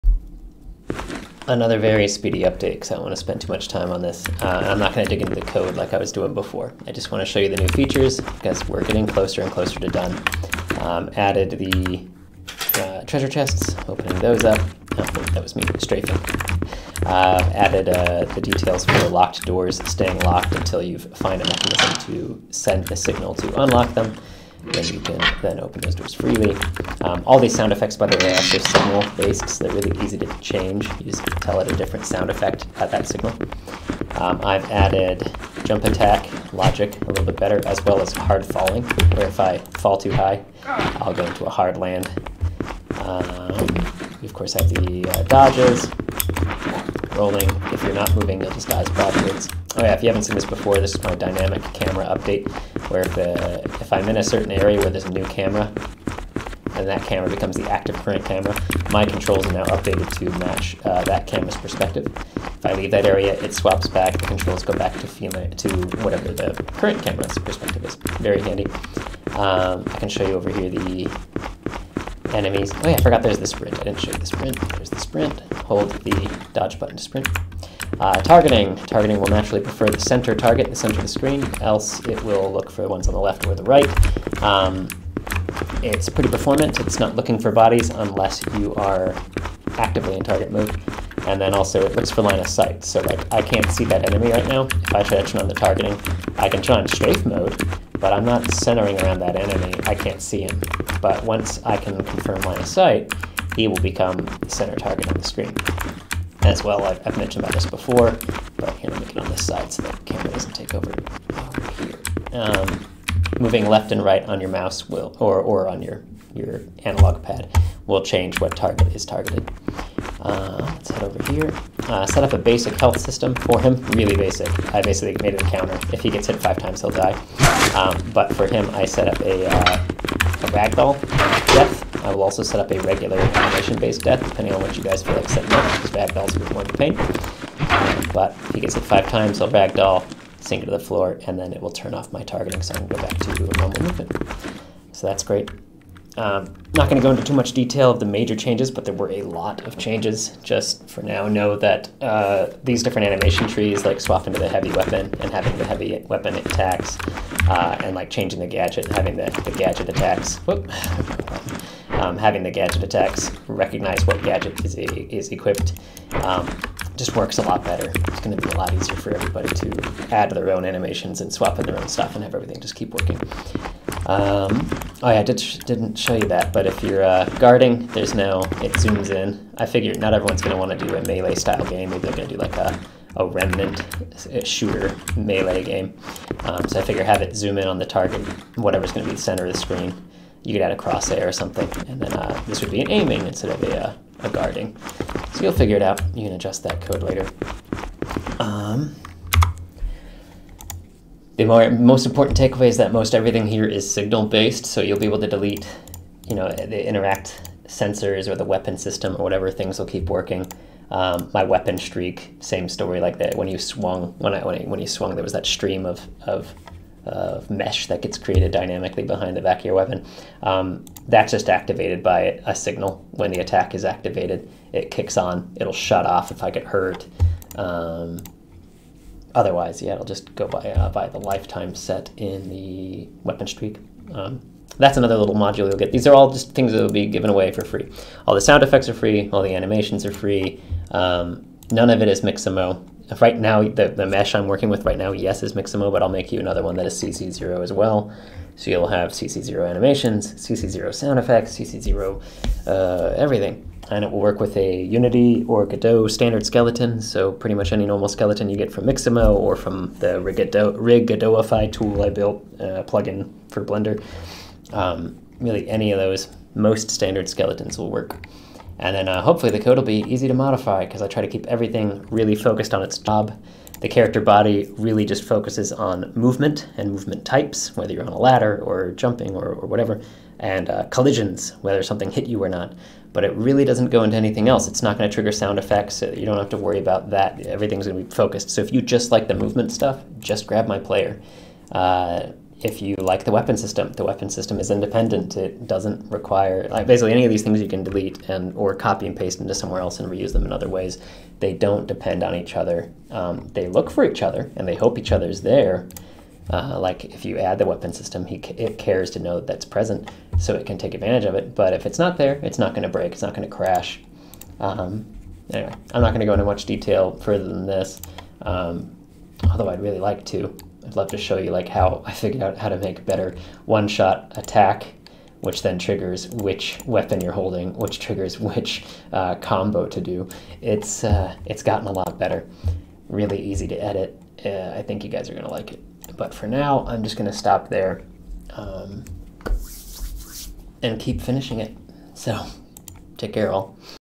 Godot 4 DevLog - SoulsLike template progress, nearly done
Massive progress these past few days, getting very close to satisfied for at least an initial release to the asset library. The culmination of 6 months of deving, testing it in game jams, finding ways to make it easy to use and easy to add to. ALMOST DONE!